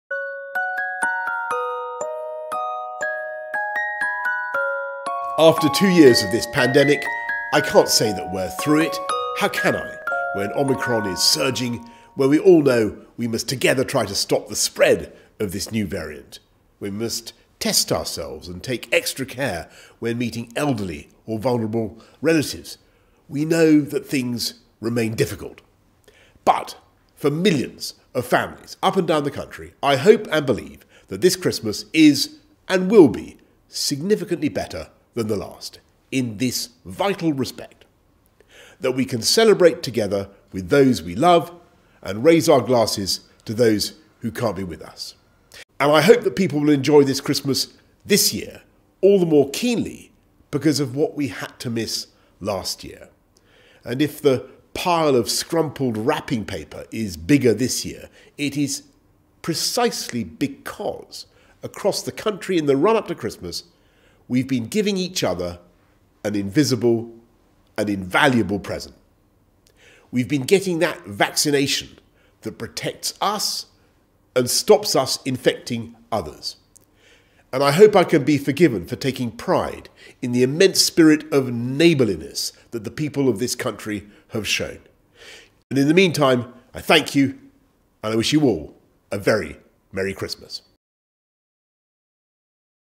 [한영자막]영국 총리의 크리스마스 메시지
영국의 총리 보리스 존슨의 크리스마스 메시지를 보며 영어 공부해 보아요~